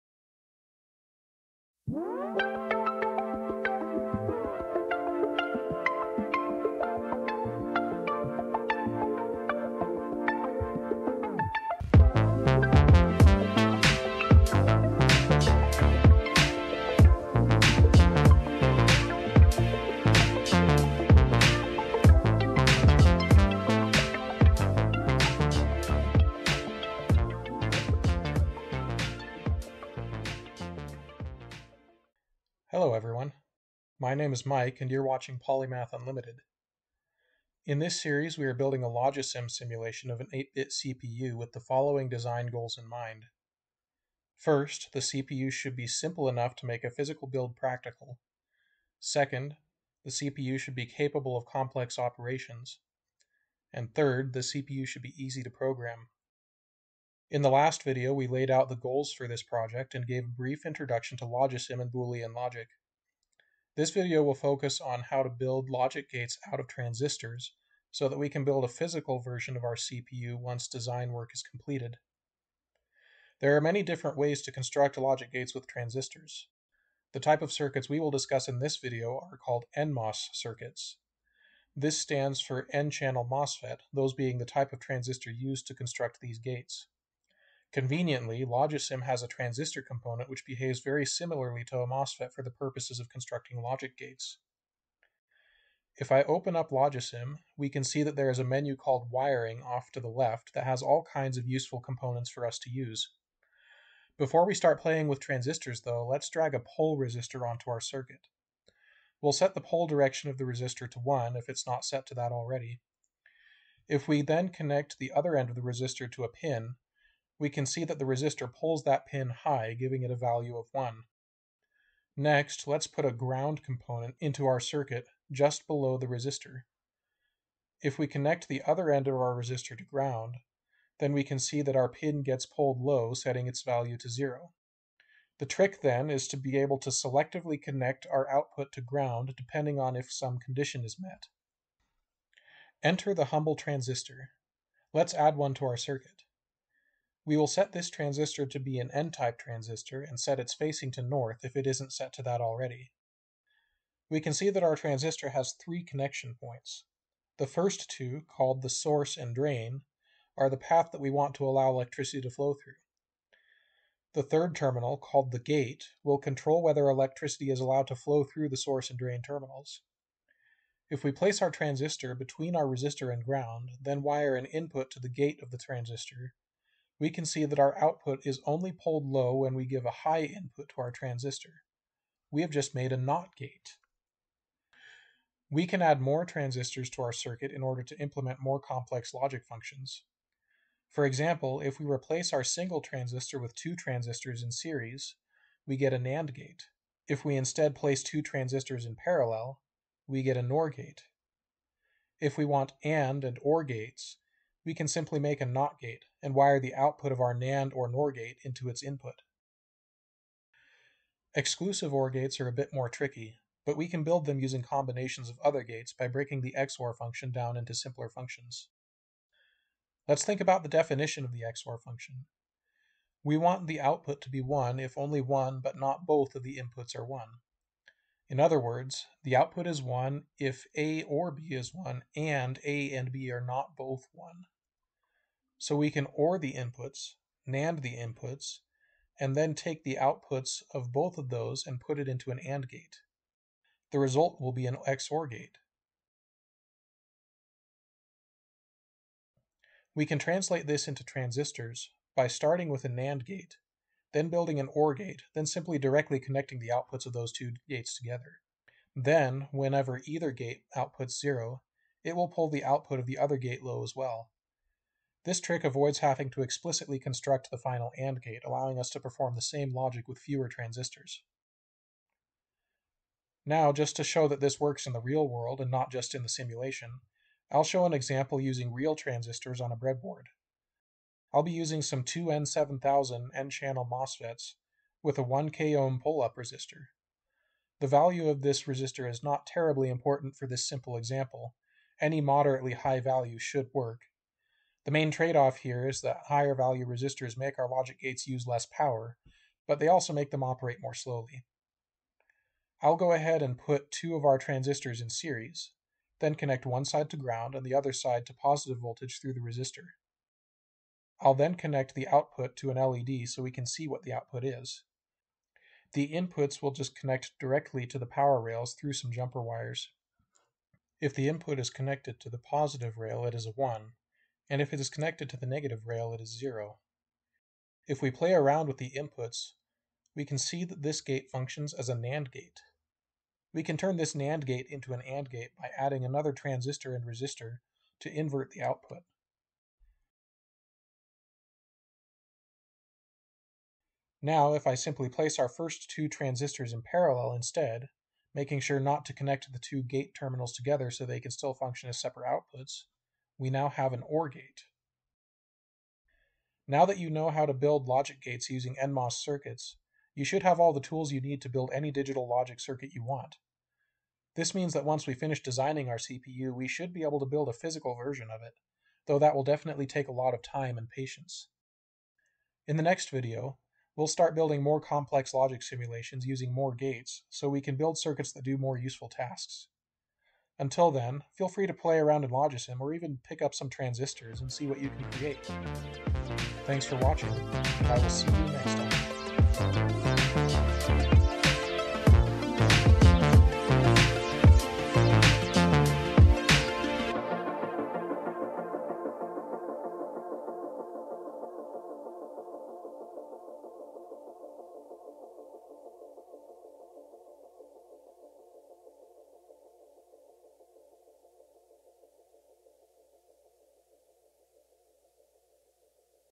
How to Build NMOS Logic Gates (The CPU You Can Build, Ep. 2)
Can you build a computer CPU from scratch?  Yes!  In this series, we are building a Logisim simulation of an 8-bit CPU using logic gates.  If this series gets enough interest, then I will also plan on building a physical version of this CPU out of real transistors in the near future.

In this episode, we will explain how to build NMOS logic gates from transistors.  This will enable us to make a physical build of our CPU once design work is done.